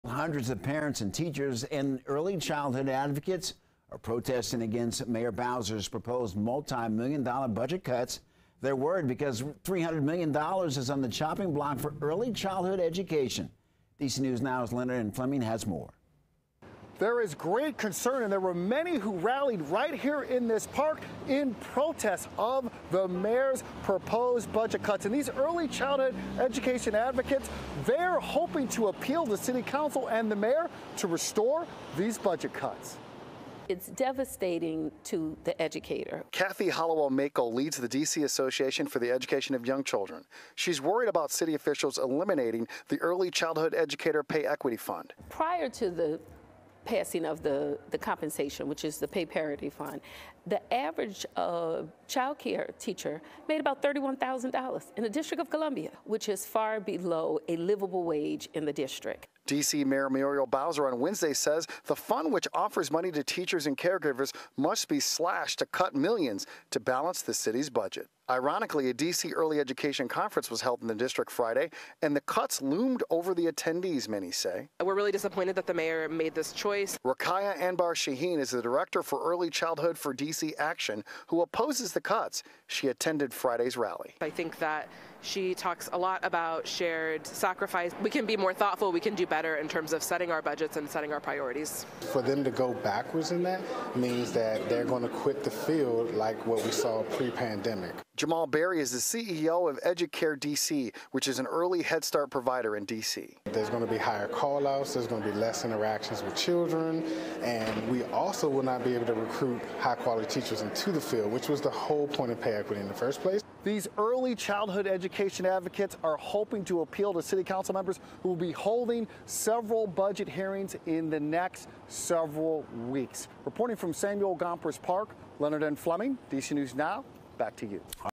Early childhood education advocates rally against DC mayor's budget cuts
Advocates for early childhood education rallied on Friday in protest against D.C. Mayor Muriel Bowser's proposed slashing of the District's pay equity fund that many fear will drive needed teachers away.